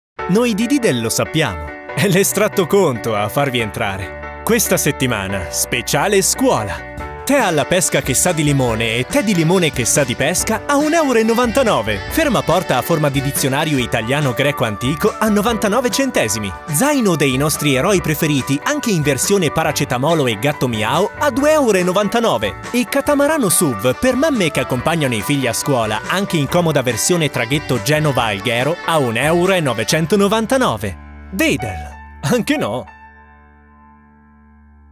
Diddle Speciale Scuola spot 2023 - parodia Lidl
Ehi scuolanti! Pronti per il ritorno al scuola con le offerte per lo speciale rientro a scuola Diddle?

Ringrazio Leonardo Manna per la realizzazione del riadattamento musicale dello spot. Qui il suo canale YouTube